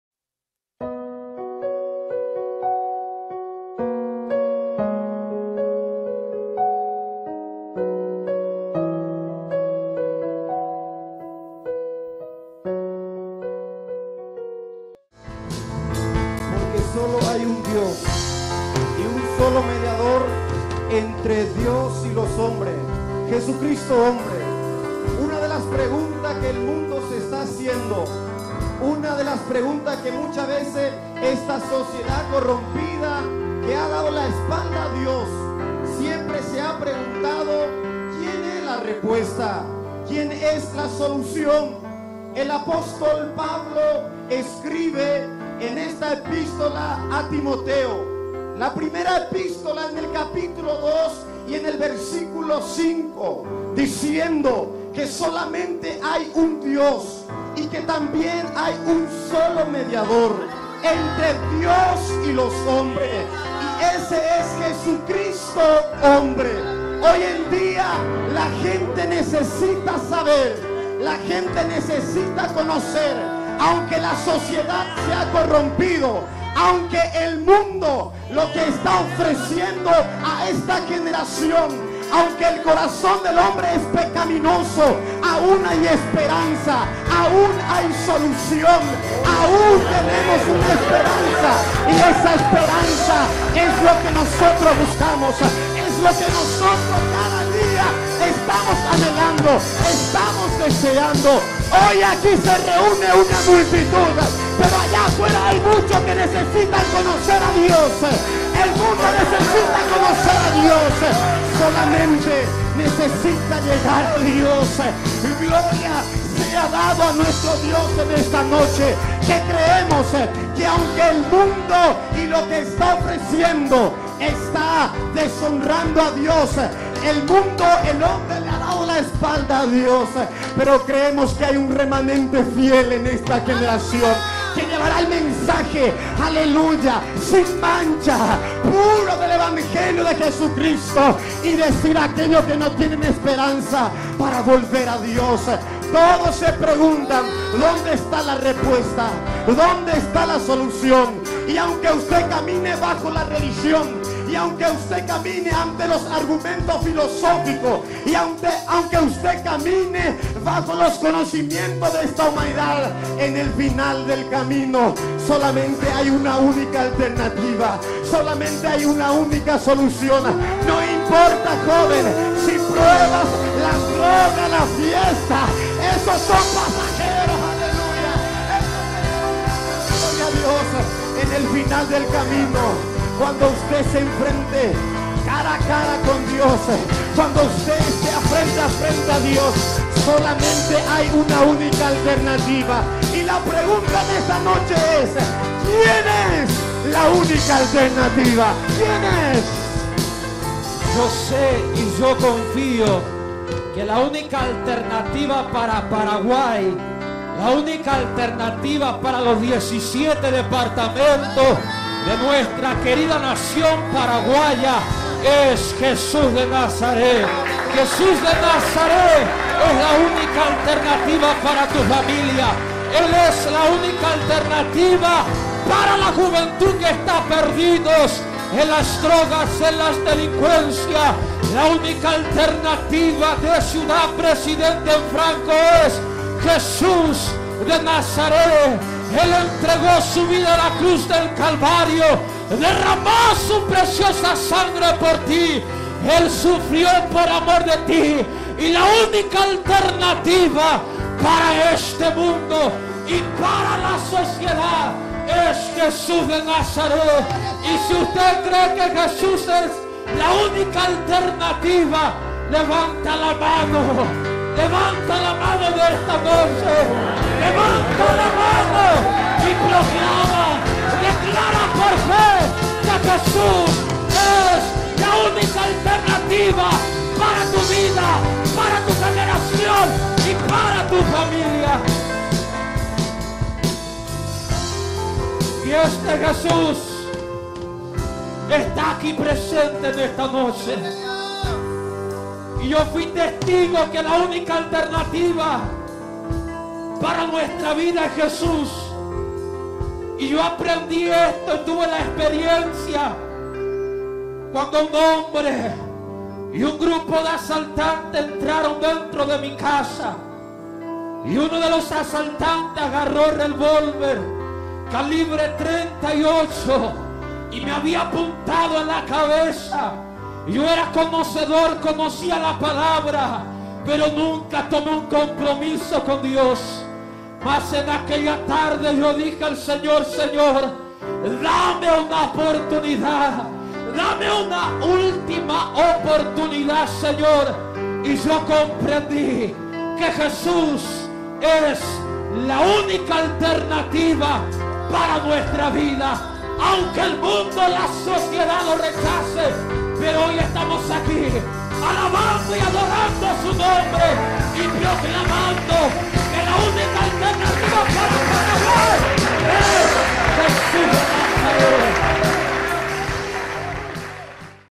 Jesús, La única Alternativa. Uziel Ocampos y Jonathan Fernandez
Mensaje de Salvación durante el lanzamiento discográfico del grupo Misión Vida.